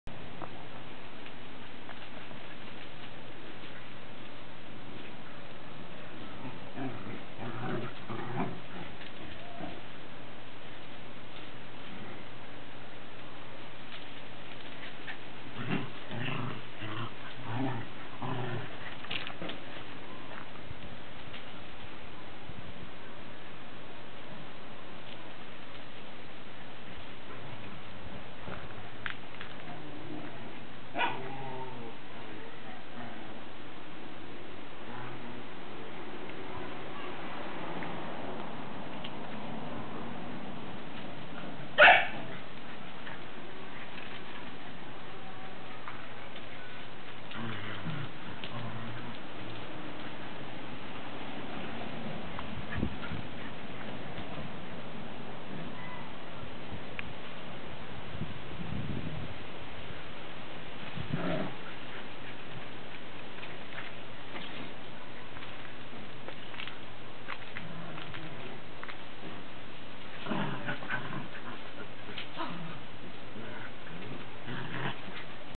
my three girls !
Basmati, Vénus & Maya playing together !